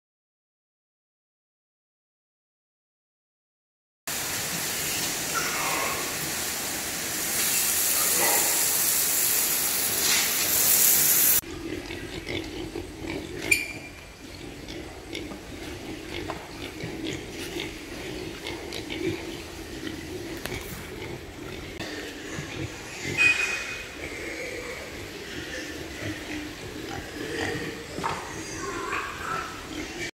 របៀបធ្វើអនាម័យអោយបានស្អាតល្អ How to hygiene on the pig
ការចិញ្ចឹមជ្រូកសំខាន់ណាស់ក្នុងការធ្វើអនាម័យ កាលយើងអនាម័យបានស្ងួតស្អាតល្អស្ថានភាពក្នុងគ្មានក្លិនឈួល (NH3) បាក់តេរី និងវីរុសក៏ជួយកាត់បន្ថយដែរ។ អនាម័យល្អ ជ្រូកមាមសុខភាពល្អ